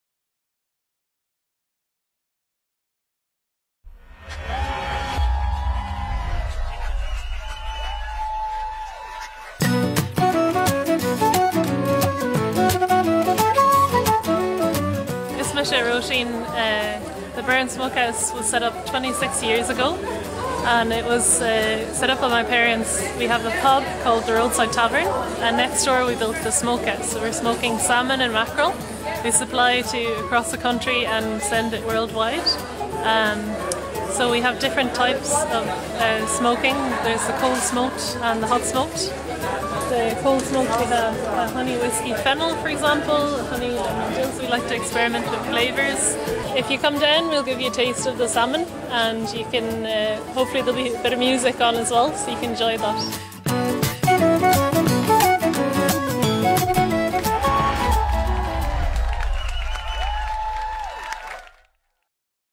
Burren Smokehouse - Róisín Curtin from the Burren Smokehouse on Fleadh TV
Róisín Curtin from the Burren Smokehouse talks to the Fleadh TV crew during the Fleadh Cheoil na hEireann Inis 2017. The Burren Ecotourism was represented by the Burren Food Trail members to highlight the culinary delights that are being produced in the Burren.